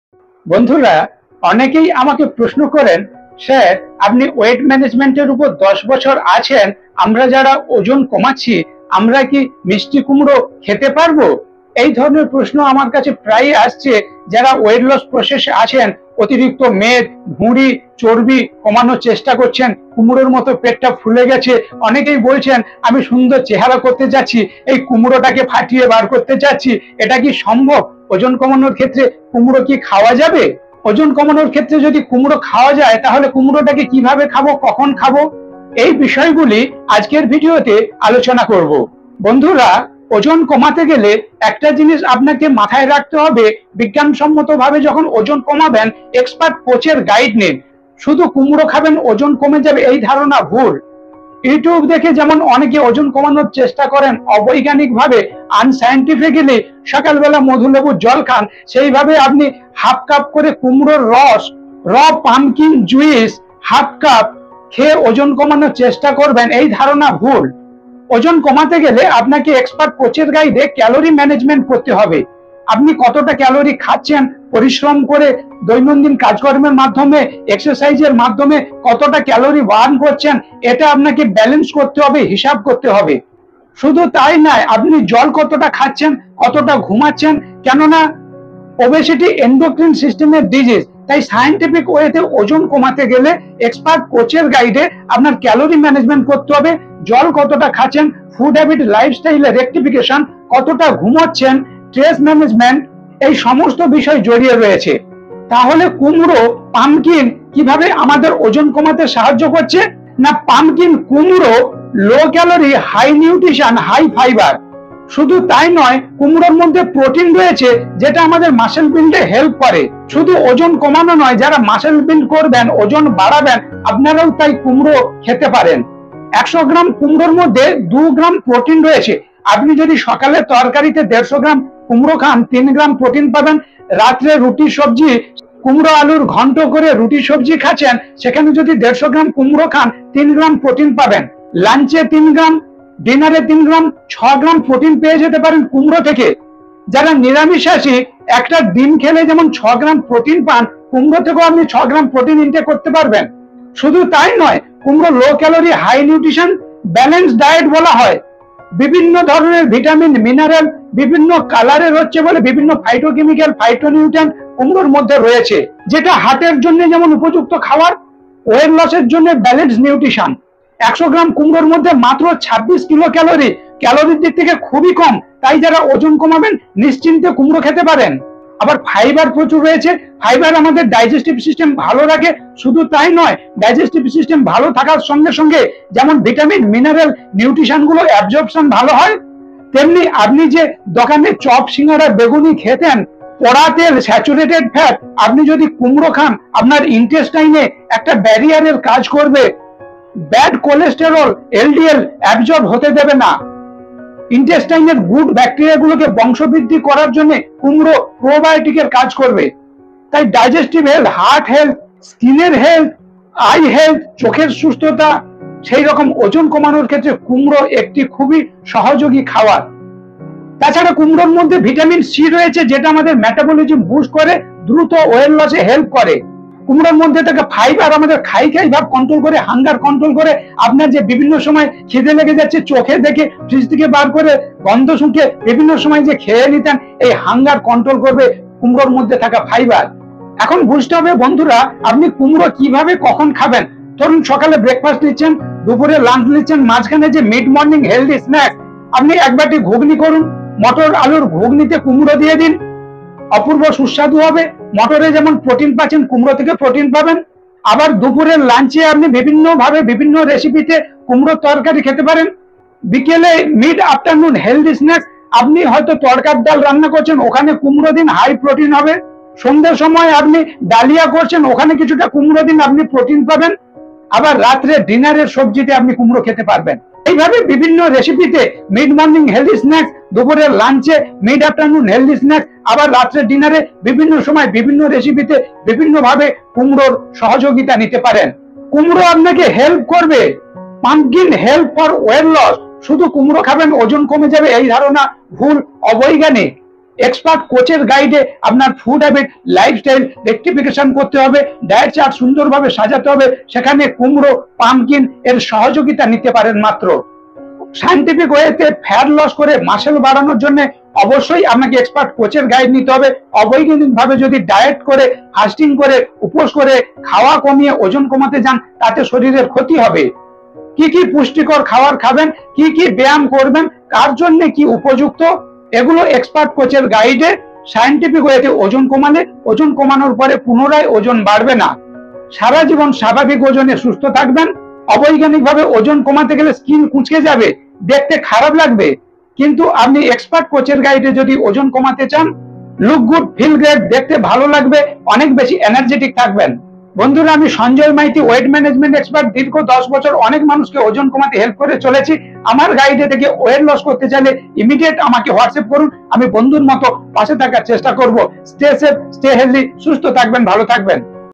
PUMPKIN কুমড়ো কিভাবে খেলে দ্রুত ওজন কমাতে পারবেন
Is pumpkin helps to lose weight ? @sanjoyfitcoach_

Pumpkin is low in calories and high in fiber , which may help with Weight Loss . Fiber can help you feel full , reduce appetite and food intake , encourage regular bowel movements , reduce blood sugar and cholesterol levels and improve mineral absorption in the gut , all of which may benefit weight management .

You can add pumpkin to a balanced diet .

This is Health Awareness Video .
For Weight Loss need proper guidance of a Coach , calorie management , food habits and lifestyle rectification .